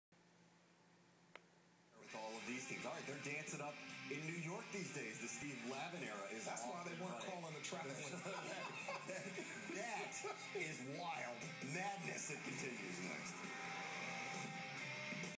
St. John's Dancing Ref - Midnight Madness 2010-11
One of the many reasons why we love "Midnight Madness" in college basketball.